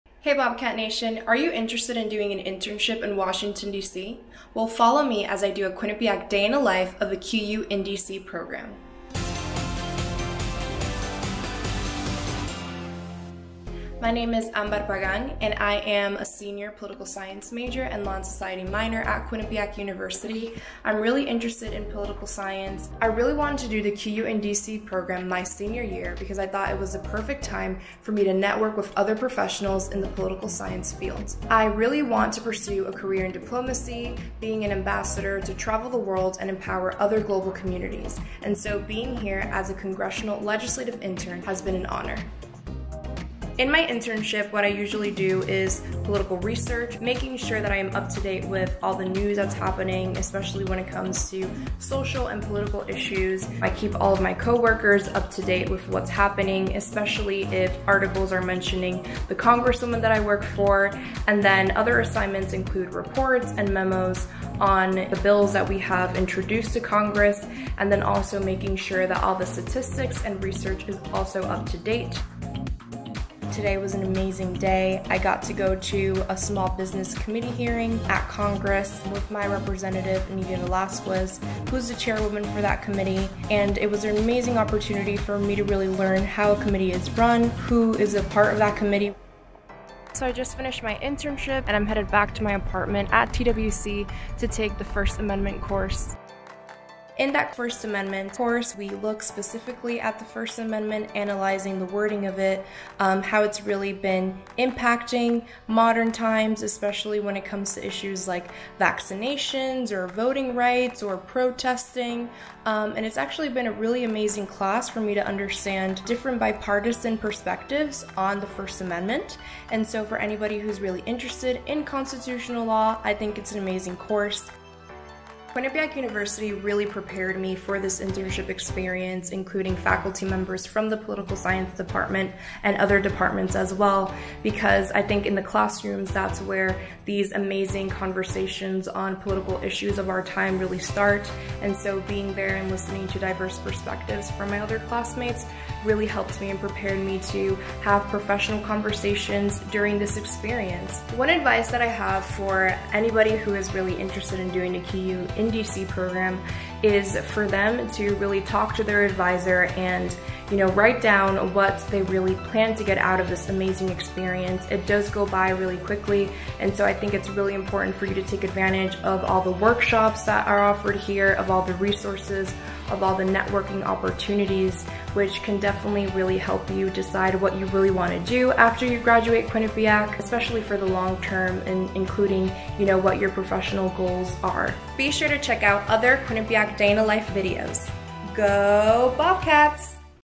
Quinnipiac Day in the Life: QU in DC Program
Are you interested in the political environment and want to intern in the heart of the United States government? Follow Ambar Pagan '22, a political science major, as she tells you about her experience in Washington DC, working at the Capitol for Representative Nydia Velazquez.